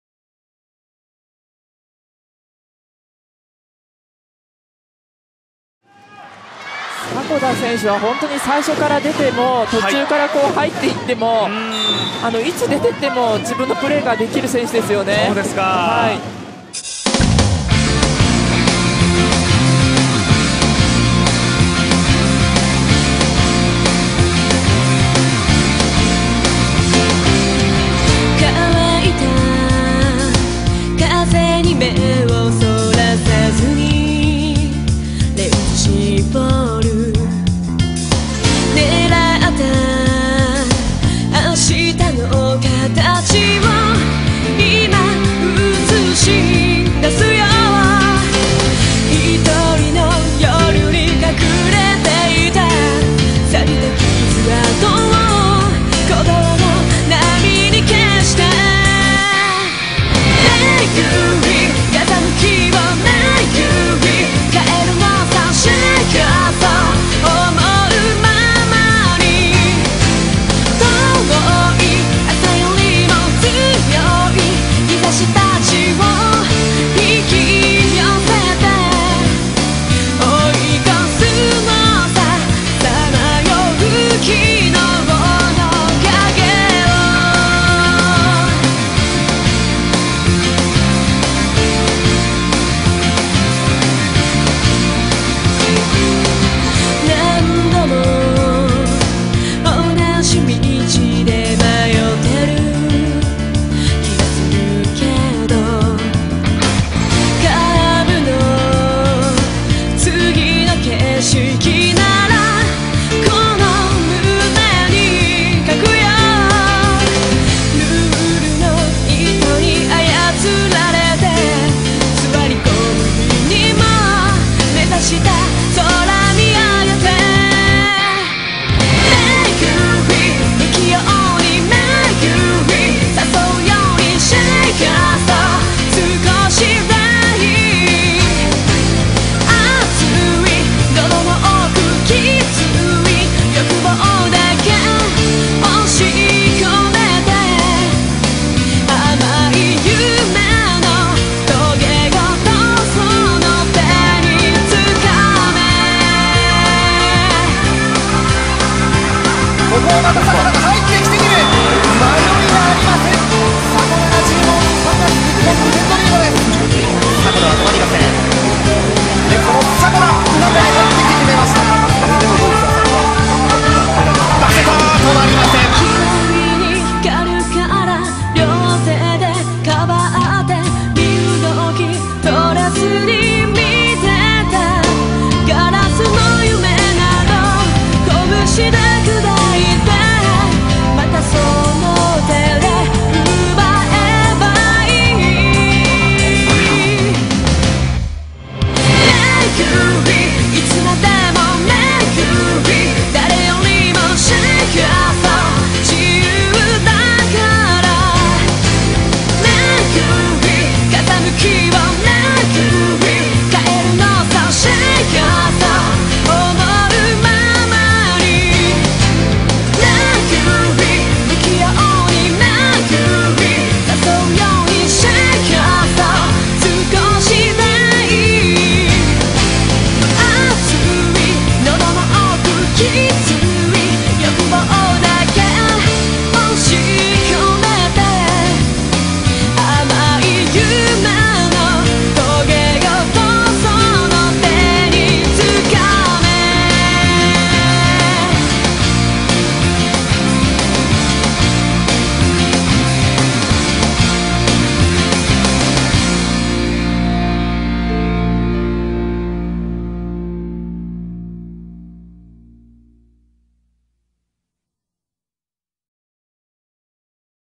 迫田さおりPV15 2012WGP〜回想2〜Saori Sakoda
センターからライトに回り込んでのバックアタックが超絶カッコいいです！対空時間もビックするくらい長いですね〜  W SAORはこの頃からやってたんだ（笑）